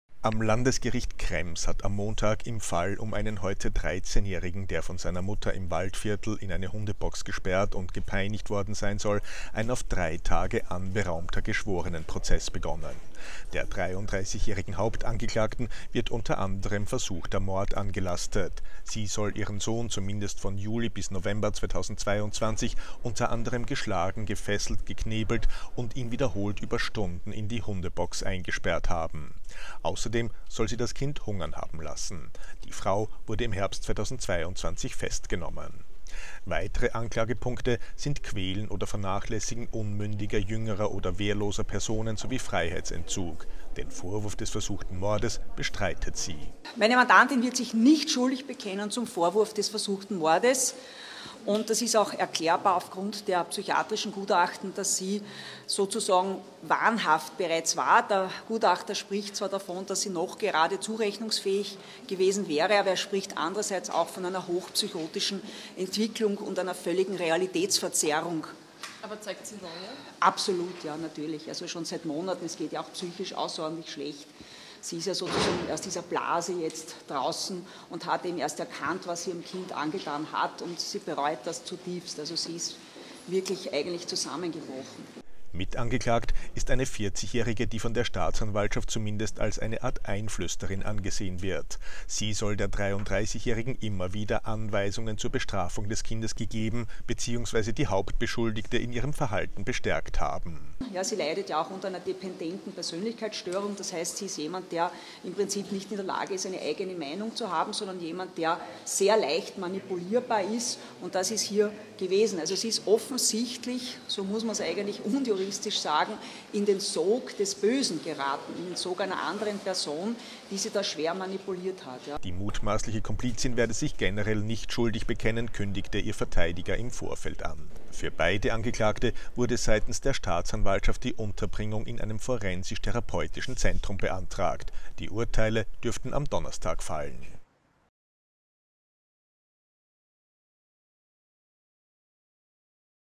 Mutter sperrte eigenes Kind monatelang in Hundebox
Das Martyrium eines heute 13-jährigen Buben, der von seiner eigenen Mutter im Waldviertel in eine Hundebox gesperrt, geschlagen, gefesselt und über Monate hinweg gepeinigt worden sein soll, wird drei Tage lang im Zentrum des Geschworenenprozesses in Krems stehen.